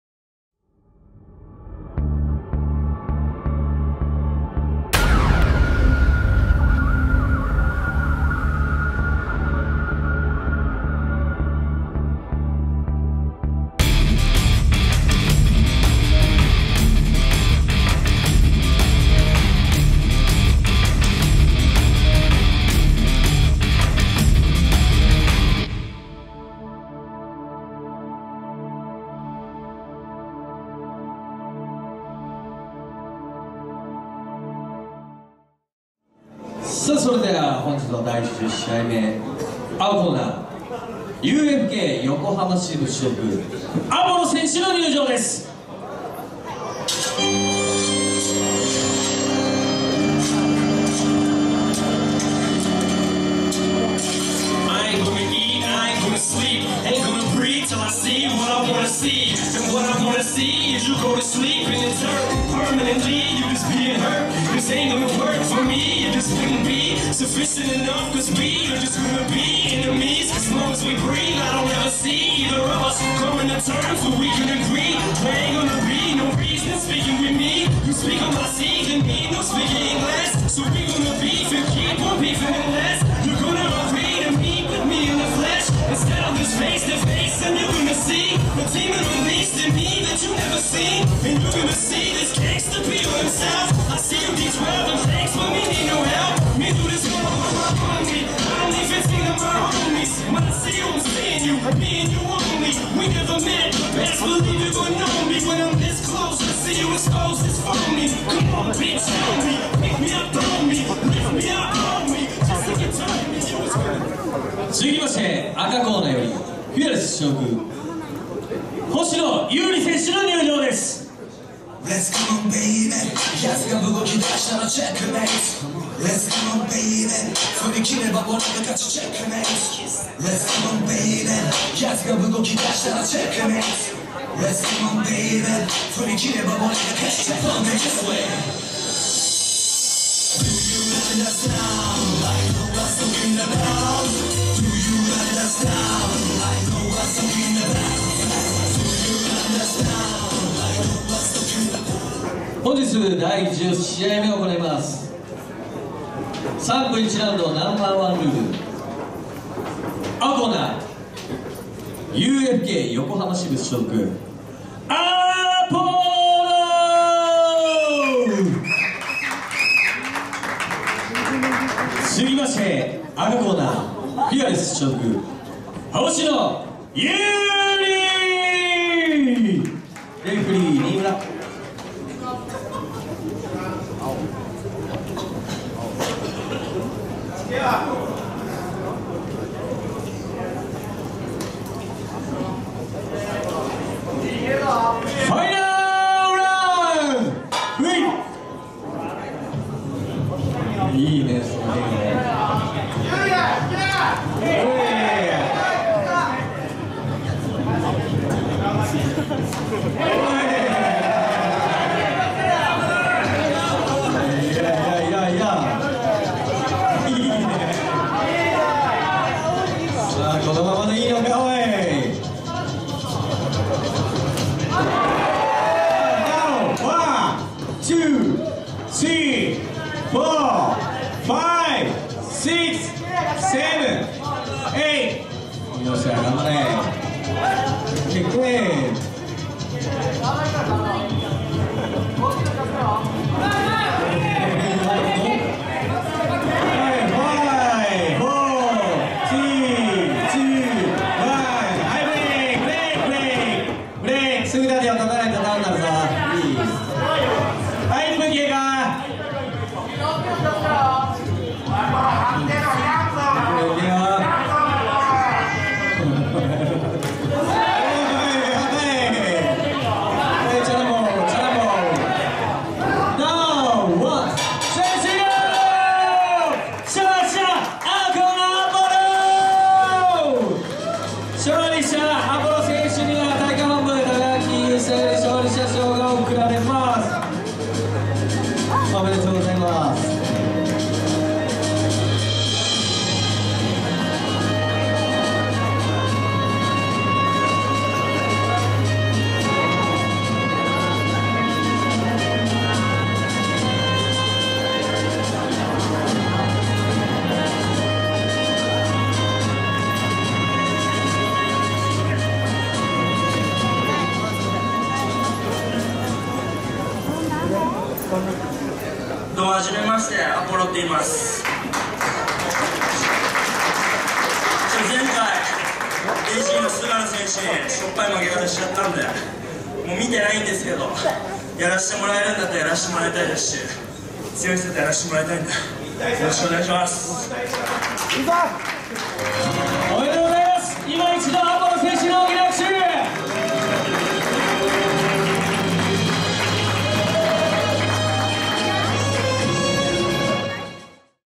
Fearless《星野裕理》vs UFK横浜支部《Apollo》〜Number1 vol.11 第十試合〜
【Number1 vol.11 第十試合】
星野裕理（Fearless） vs Apollo（UFK横浜支部）
会場：新木場1stRING
開催：2016年1月17日（日）